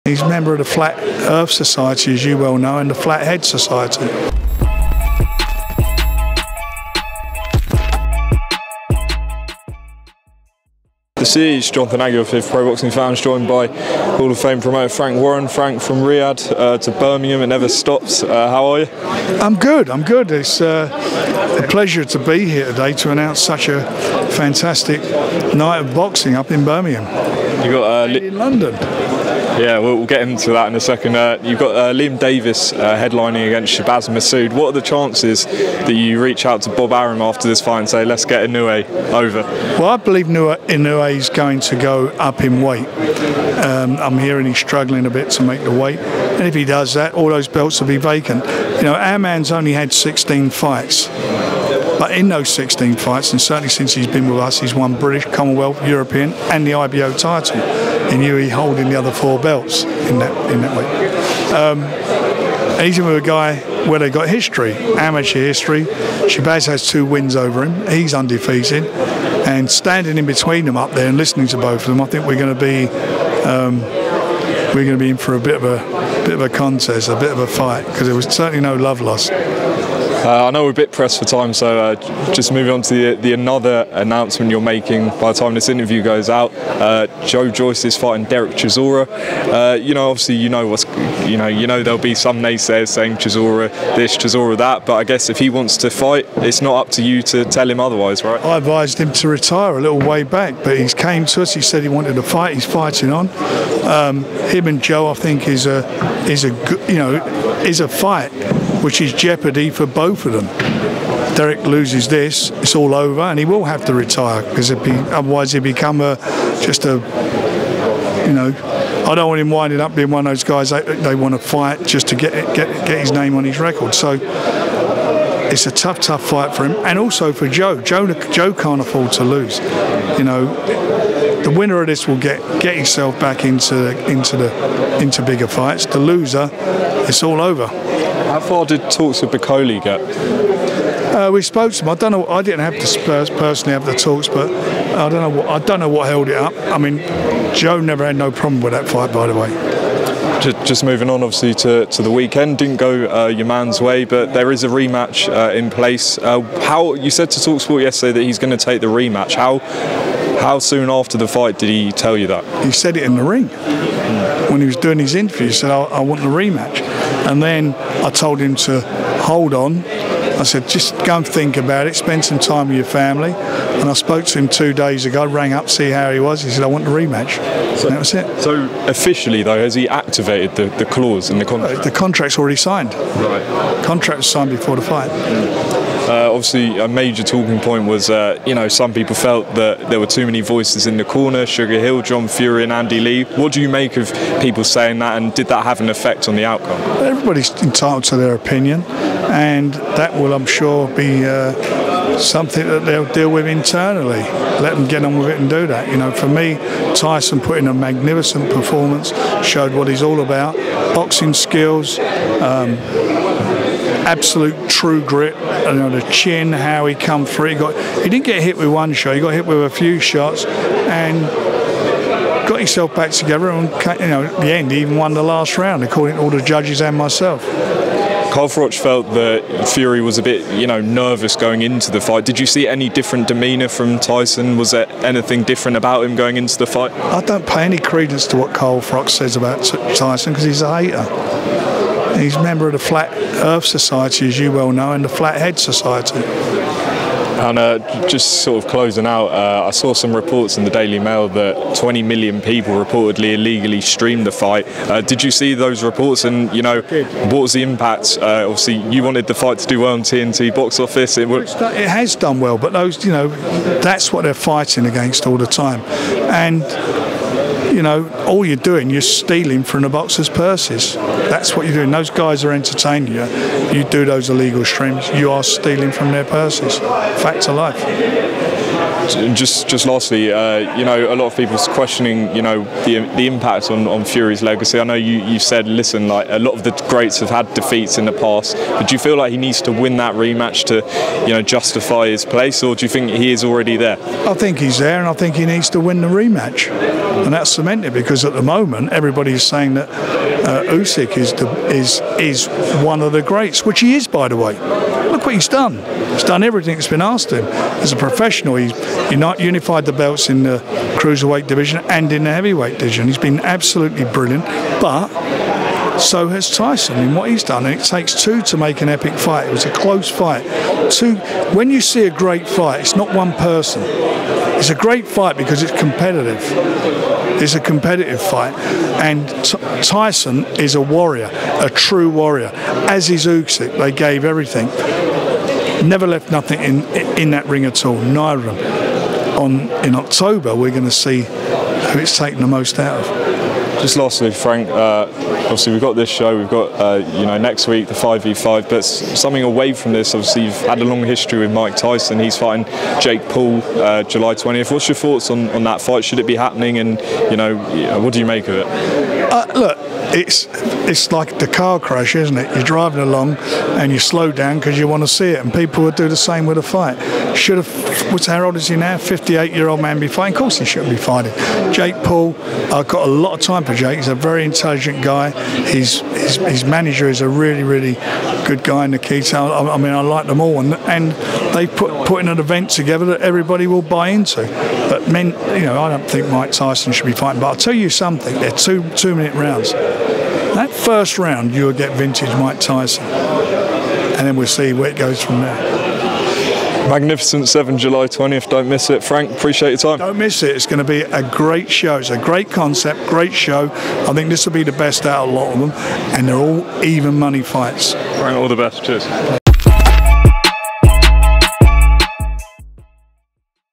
TOO MANY VOICES IN TYSON FURY CORNER? - FRANK WARREN RESPONDS, BLASTS "FLAT HEAD" CARL FROCH, JOYCE
Frank Warren spoke to us at the press conference for his Magnificent 7 card on July 20, Joe Joyce vs Derek Chisora, hits back at Carl Froch, responds to suggestions that Tyson Fury had too many voices in his corner during his fight with Oleksandr Usyk. Check out the video!